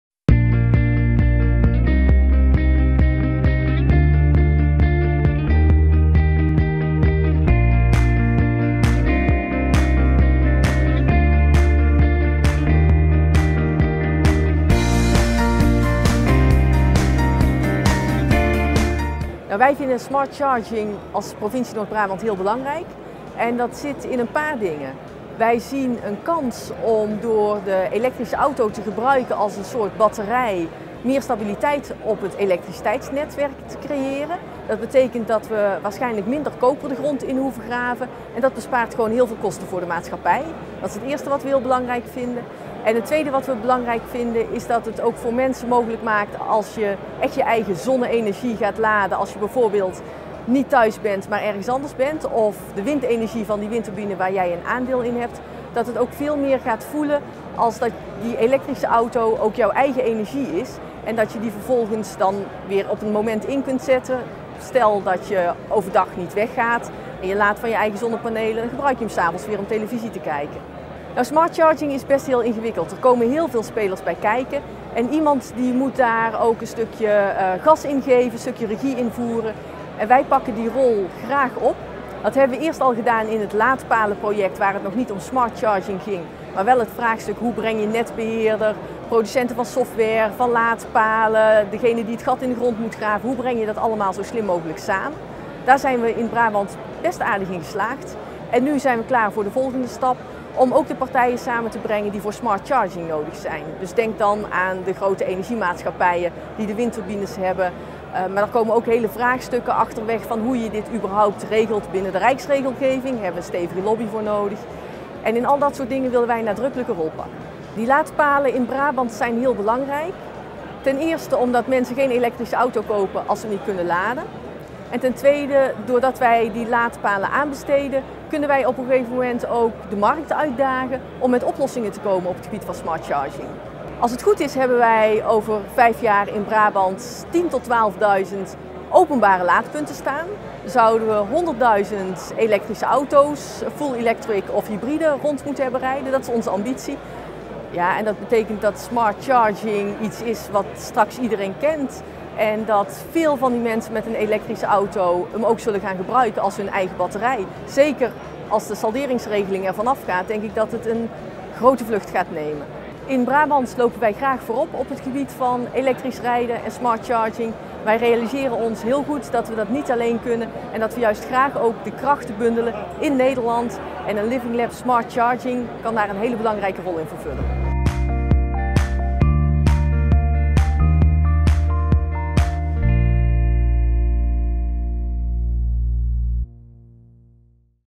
Anne-Marie Spierings, gedeputeerde provincie Noord-Brabant
Gedeputeerde Anne-Marie Spierings van de provincie Noord-Brabant toont zich visionair en als doorpakker op het dossier elektrisch rijden. Meer specifiek ook op Smart Charging. Bekijk het interview dat we met haar hielden tijdens Ecomobiel 2016.

Duurzame stroom komt in pieken en dalen: de wind waait en de zon schijnt op momenten die de elementen voor ons uitzoeken. En dat is niet altijd op het moment dat we die stroom ook nodig hebben. Die stroom moeten we dus opslaan om deze voor 100 procent te benutten. De accu’s van elektrische auto’s zijn daarvoor een perfecte oplossing. Hoe dit in de praktijk werkt, wordt onderzocht, getest en toegepast in het Living Lab Smart Charging. Nederland heeft hierover veel kennis en werkt zo mee aan de internationale standaard.